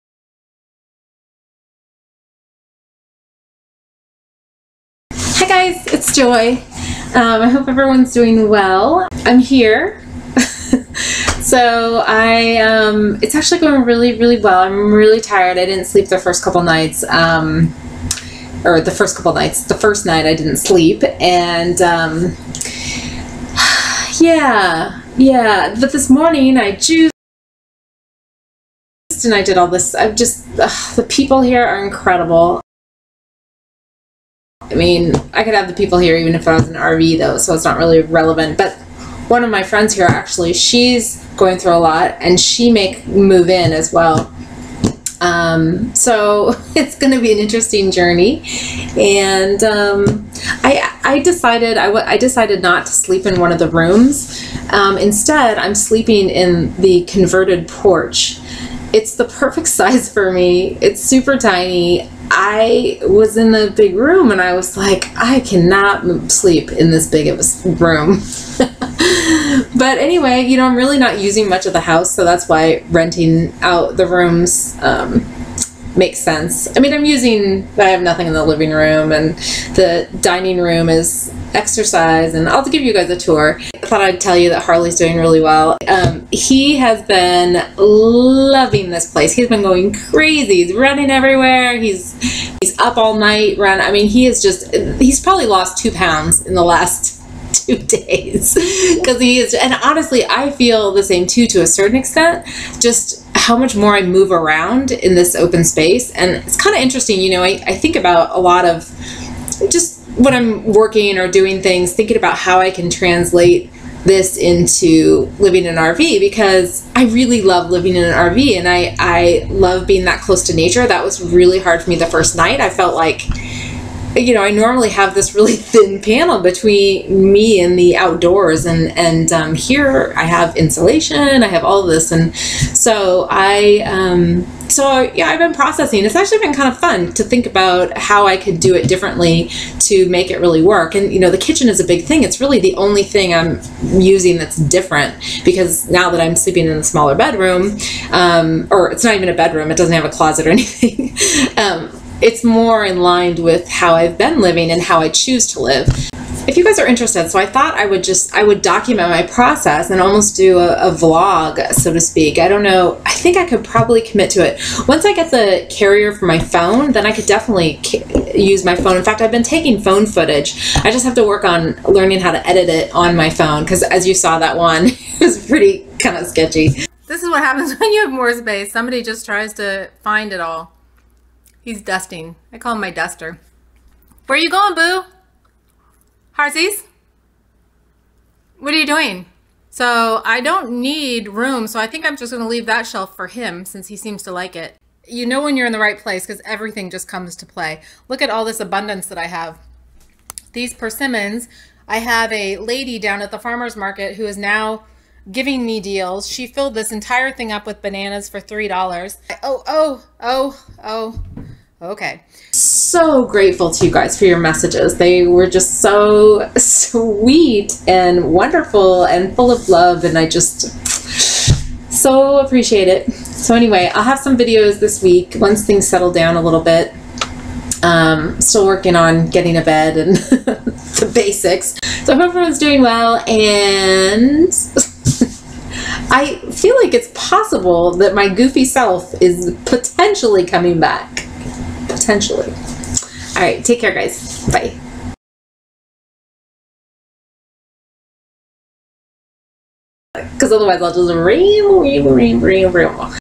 PapayaJoy 12 20 2014 - Update & Thank You!
PapayaJoy shares an update and is extremely grateful for your kind words, love and support!